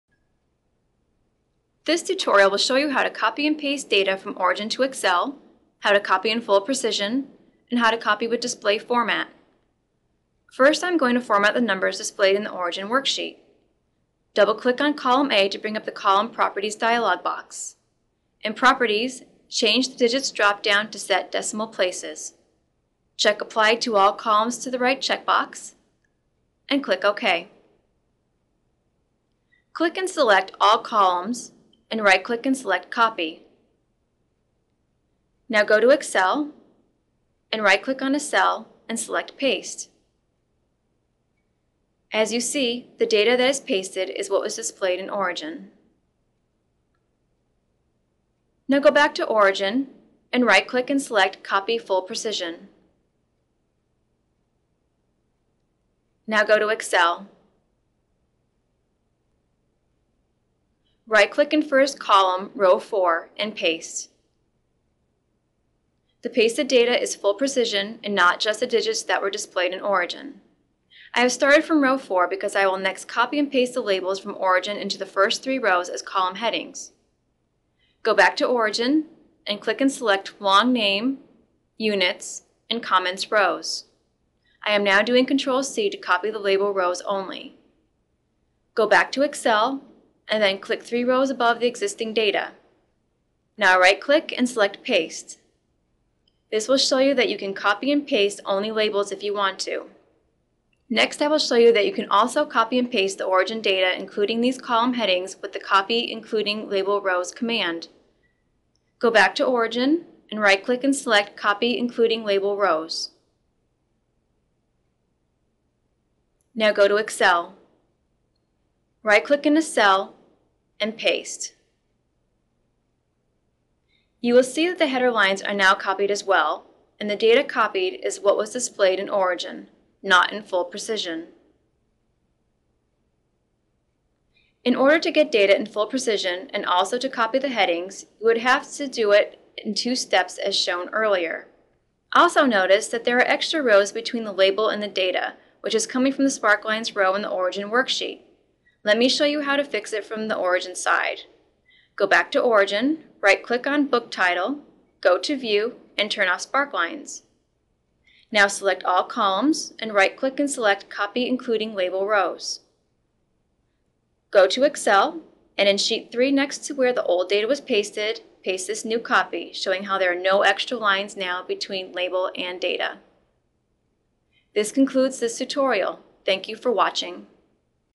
Origin 8: Copying from Origin 8 and Pasting into Excel
Learn how to copy Origin 8 data and paste it into Excel in full precision.